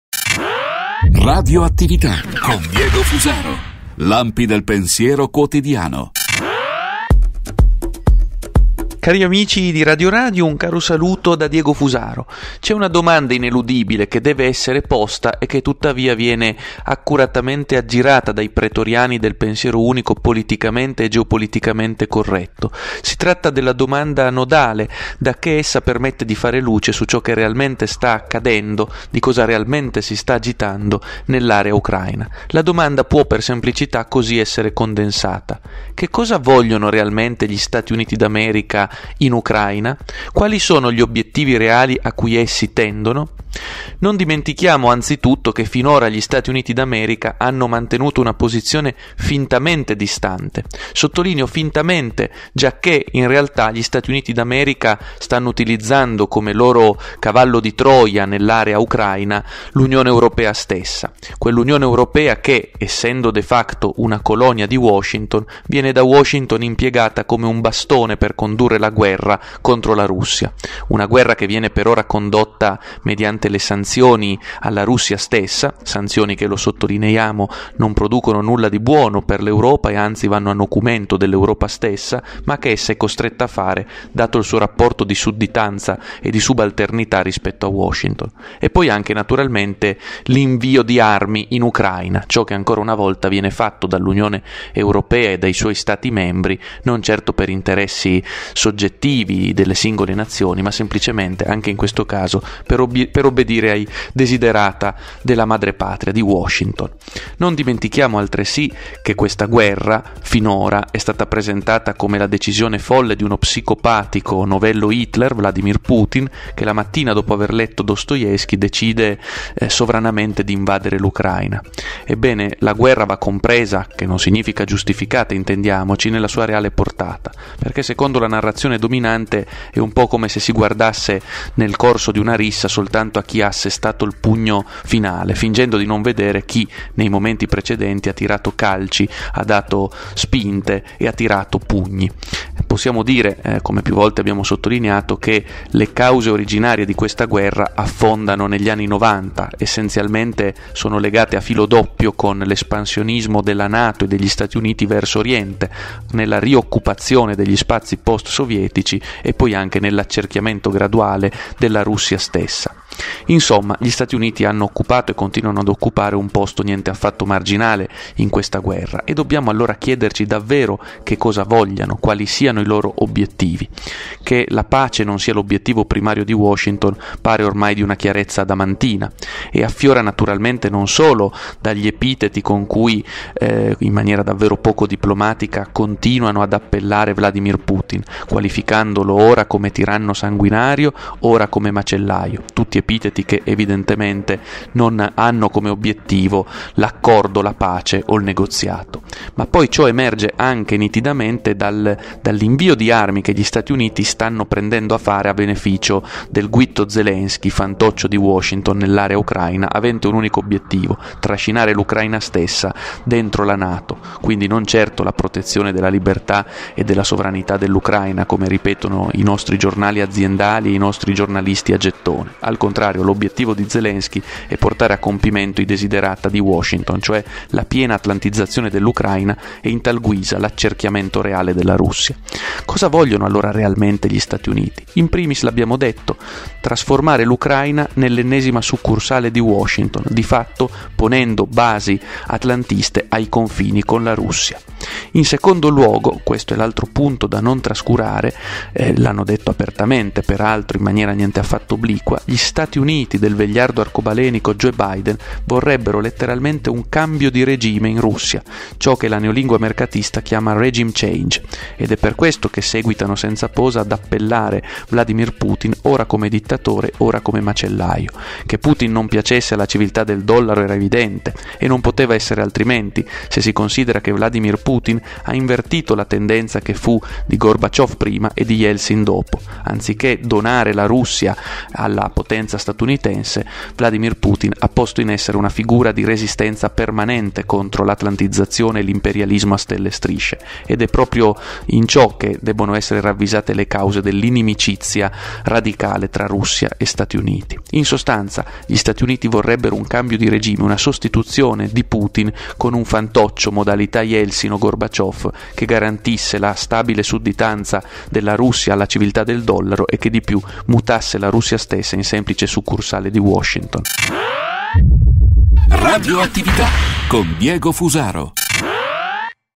Nella mente di Joe Biden: lo sfogo e quella allusione sul golpe. Il drammatico sospetto sulla guerra
C'è una domanda ineludibile e che accuratamente viene aggirata dai pretoriani del pensiero unico geopoliticamente corretto. Si tratta della domanda nodale dacché essa permette di fare luce su ciò che realmente sta accadendo nell'area dell'Ucraina. Che cosa vogliono realmente gli Stati Uniti d'America in Ucraina e quali sono gli obiettivi reali a cui essi tendono?

Non dimentichiamo che gli Stati Uniti d'America hanno mantenuto fino ad ora, una posizione fintamente distante, sottolineo fintamente giacché gli StatiUnitid'America stanno utilizzando come cavallo di Troia nell'area ucraina l'Unione europea stessa. Essendo quest'ultima una colonia di Washington, viene da impiegata da Washington stessa come un bastone per colpire la Russia.

RadioAttività, lampi del pensiero con DiegoFusaro